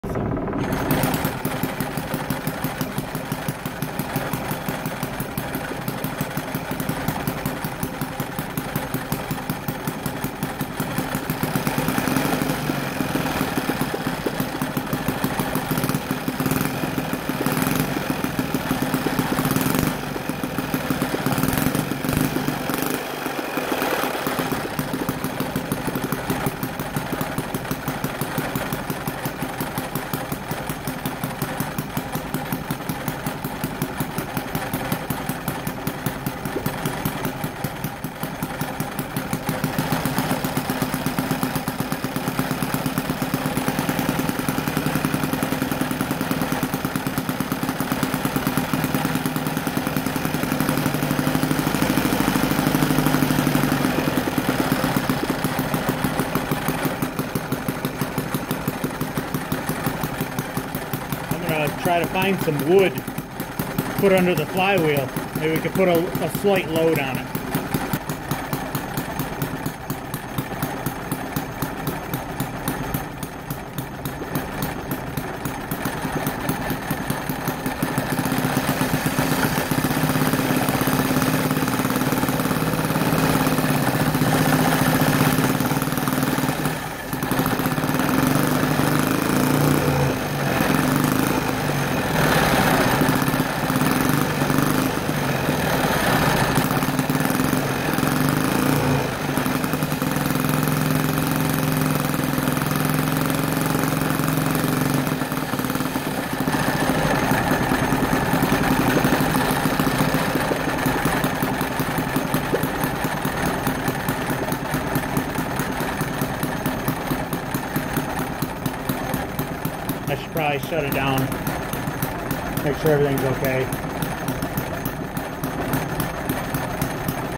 Four stroke 10cc OHV single cylinder engine. First run!
First run of my homemade four stroke 10cc over head valve engine.  This video is a continuation of a short video clip that was posted on instagram.

A little bit about the engine.
This engine is made from bar stock. The base that also serves as the oil sump is cast aluminum using a wood pattern. Other than the connecting rod,  fasteners and a few E-clips, every component is made by hand and/or on older manual machines.

The connecting rod was my first part designed with Fusion 360 and using its cam function was able to machine the rod on my cnc mill.

Its lubrication is splash type with the dipper connected to the rod cap. It has a full set of rings. Compression, scraper and oil ring made from cast iron.

Its bore and stroke is .875" X 1"
Ignition system is points & condenser